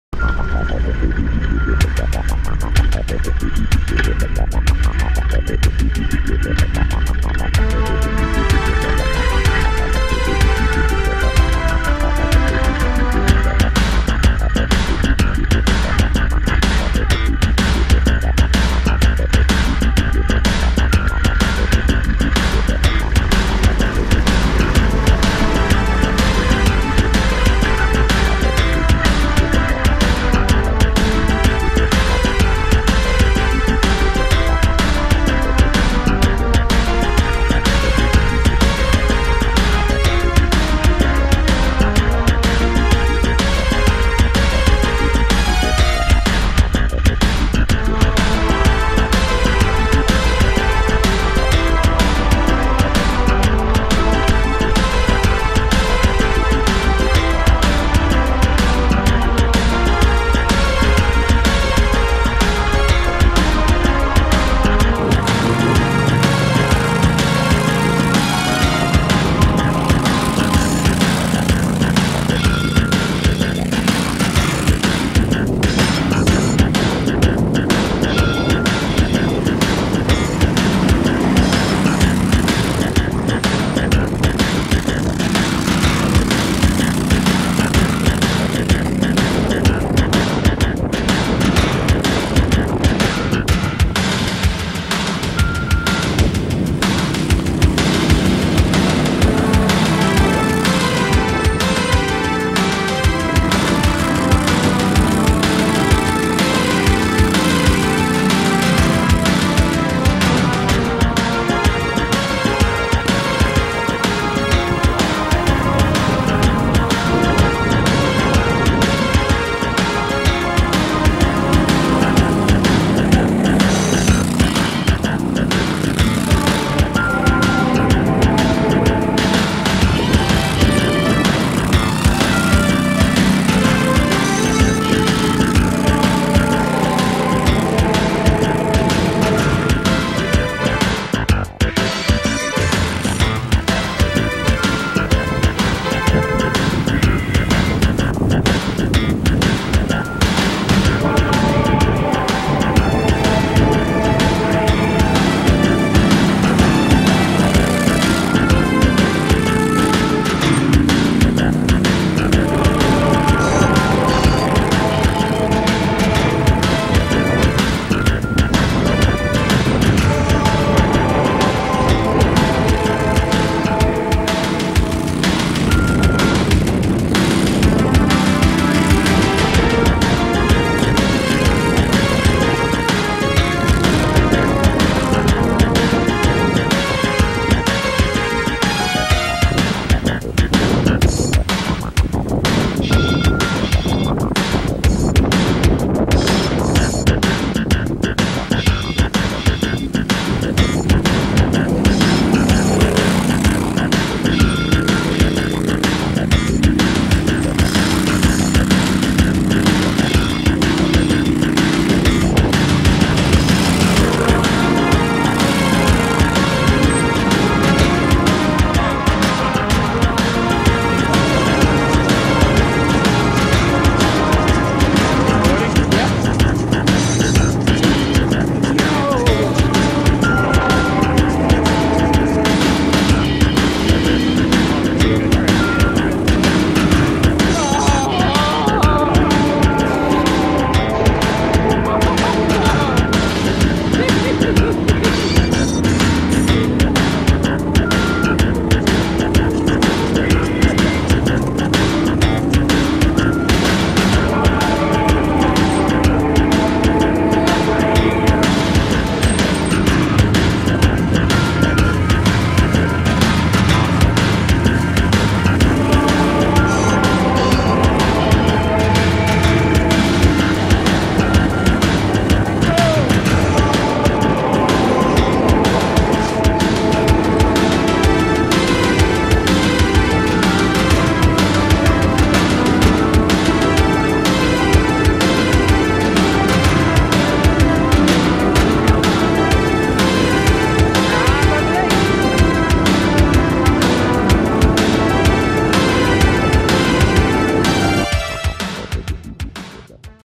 The Jawswagon after the Blizzard of 2/9/13
With about 20" of snowfall in this part of Long Island, the Jawswagon (Max II 6x6) is on a dip through the neighborhood!.. Got to test out the new HERO3 GoPro Black action cam which attached nicely to the rollbar.. The handheld HD cam was used as well... Plenty of 360's & drifts on the snow & ice covered roads!.. Getting quite a bit of smiles & "what the hells???" from the people digging out of the storm!...